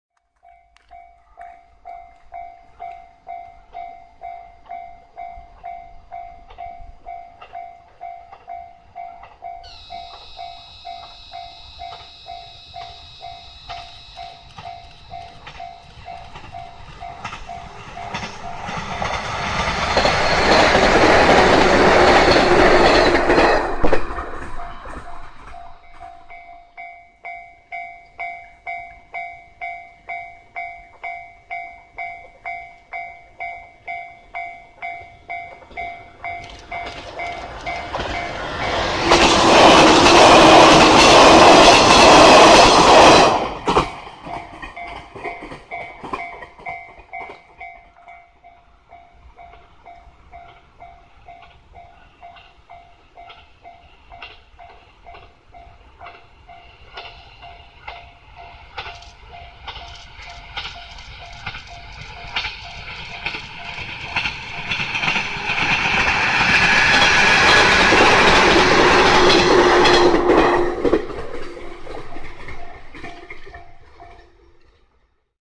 両毛線115系 思川～小山
2016年9月17日撮影
思川～小山間の田園風景の中を走る115系湘南色です。